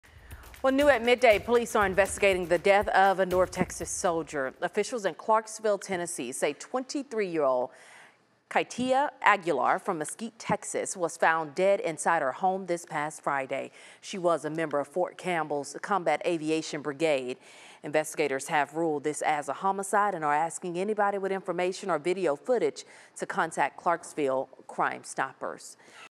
North Texas soldier killed near Kentucky Army base
Duenas-Aguilar was from Mesquite, her death has been ruled a homicide.